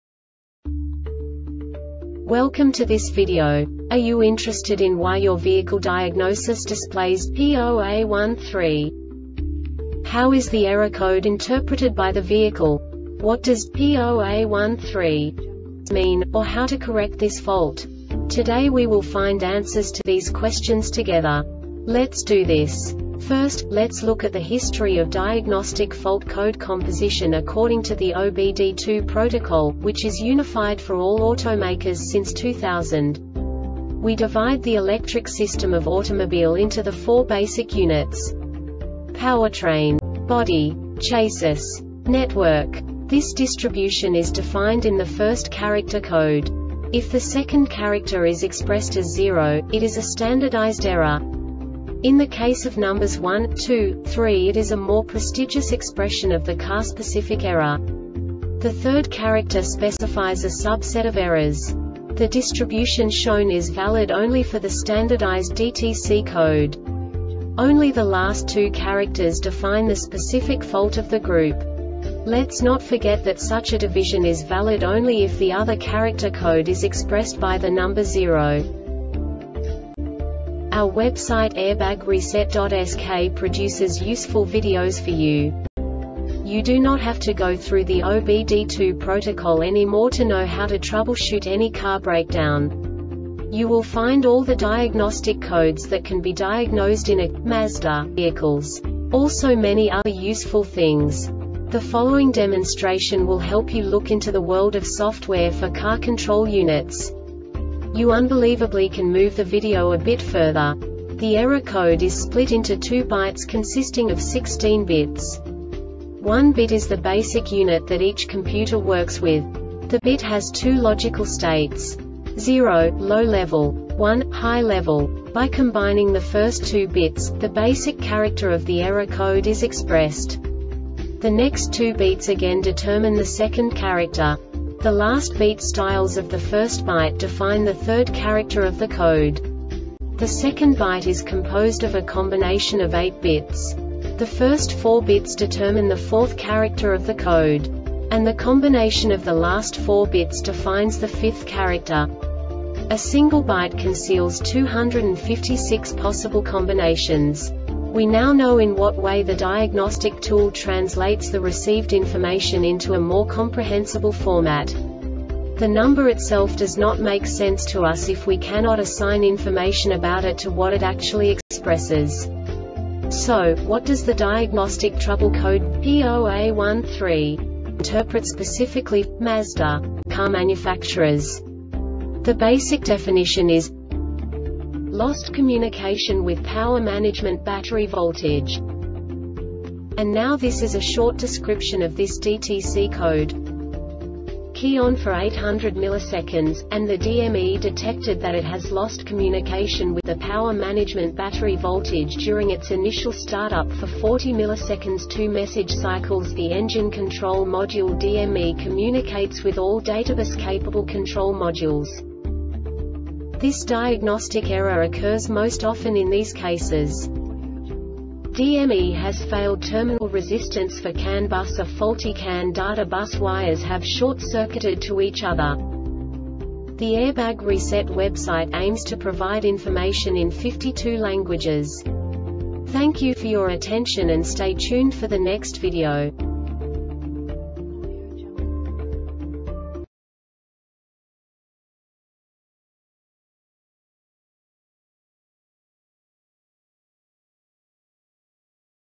DTC Mazda P0A13 Short Explanation
Contents:
0:21 Basic DTC analysis according to OBD2 protocol standard.
1:48 Insight into programming
2:58 A diagnostically understandable description of the Diagnostic Trouble Code
3:05 Basic error definition

Make:
Mazda
Code:
P0A13
Definition:
Lost Communication With Power Management Battery Voltage
Description: